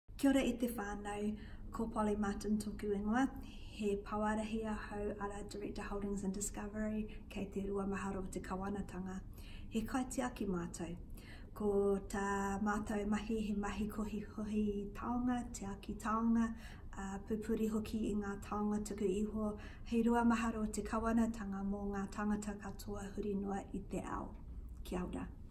Director Holdings and Discovery discusses her mahi for Te Wiki o te Reo Māori
Polly Martin is the Director Holdings and Discovery and we chat to her about her role and mahi.